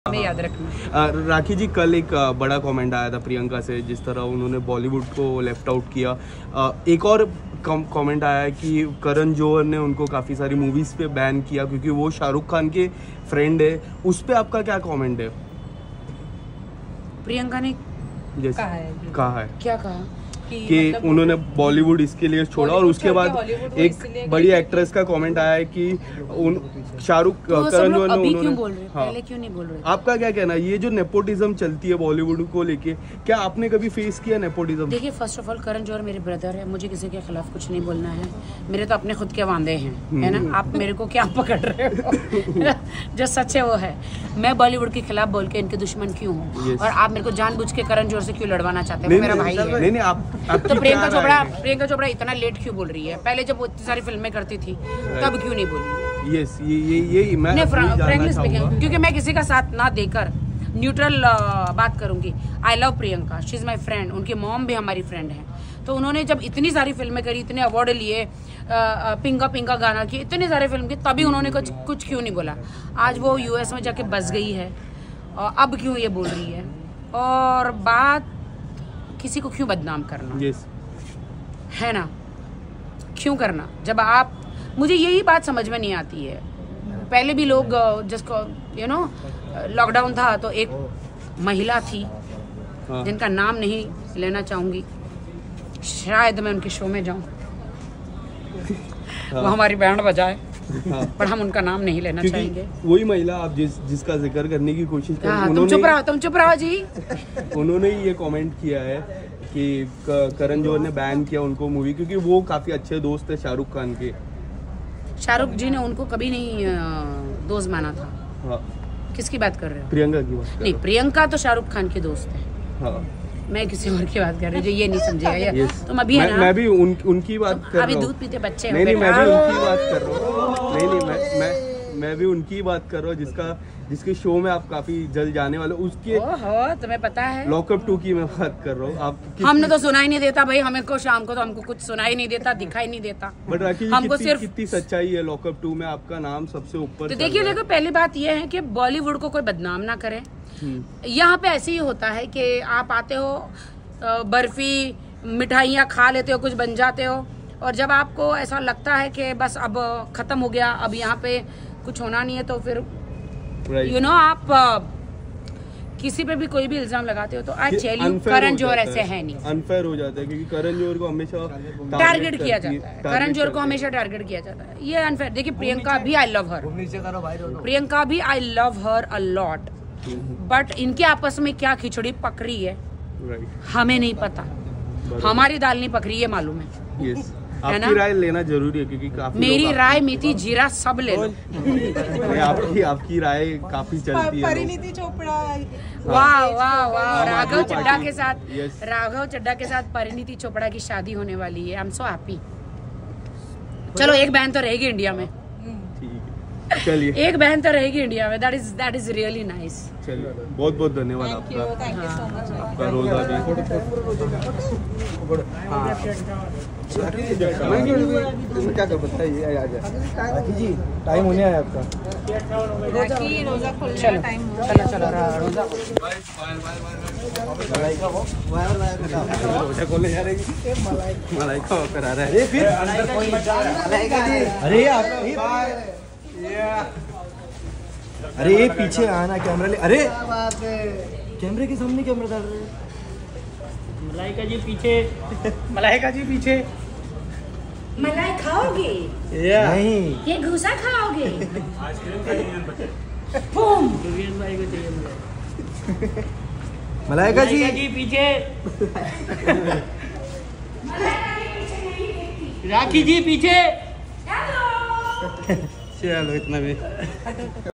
में कुछ नही बोलूंगी😜Rakhi Sawant Unexpected ReactionOn Priyanka ChopraLeft Bollywood,KaranJohar,SRK
मेरे तो खुद के वादें है में कुछ नही बोलूंगी 😜 Rakhi Sawant Unexpected Reaction On Actress Priyanka Chopra Left Bollywood Because Of Karan Johar & Sharukh Khan